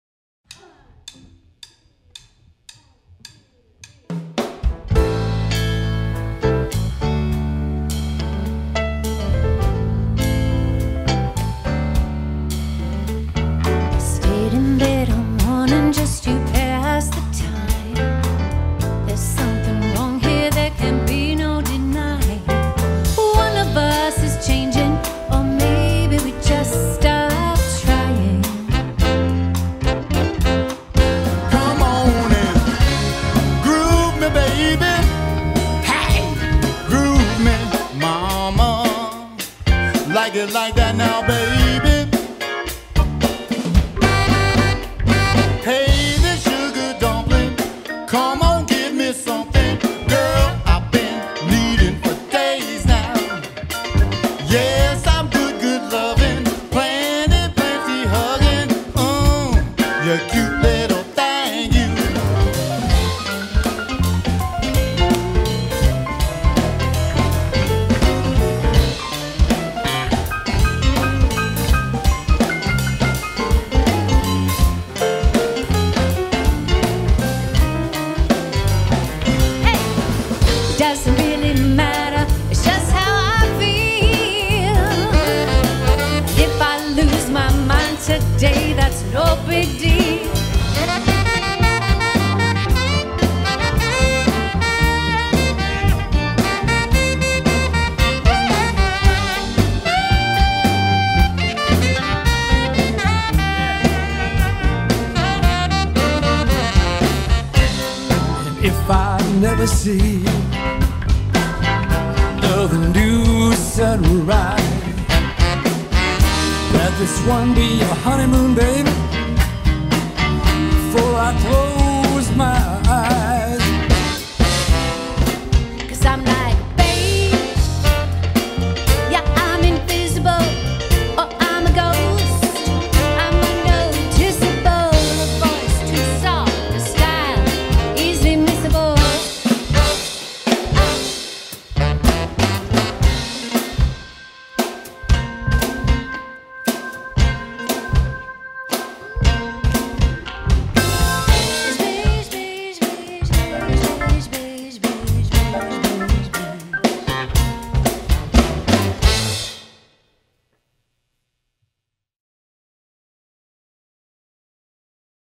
Matt Finders and Friends Promo
This is a short promo featuring Matt Finders and Friends, a six piece band featuring:
John Whitney- guitar/vocals
Karen Marguth- vocals
Noel Catura- sax
kelly Fasman- drums